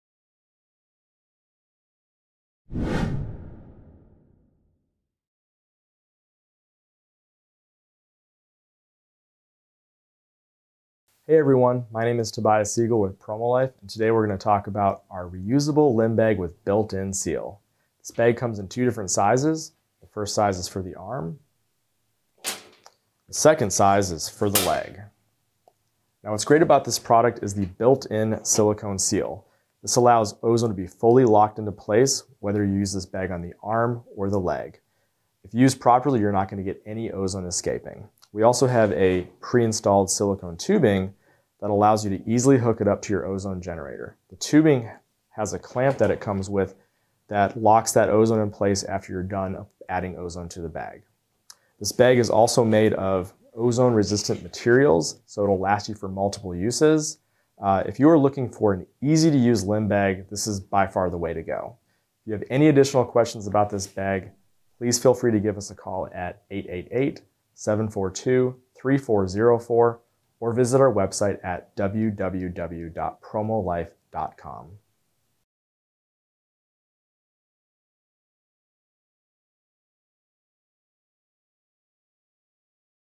Ozone Limb Bags with Silicone Seal
This quick video will show you our ozone limb bags with seal, which help seal in the ozone without the need for velcro straps.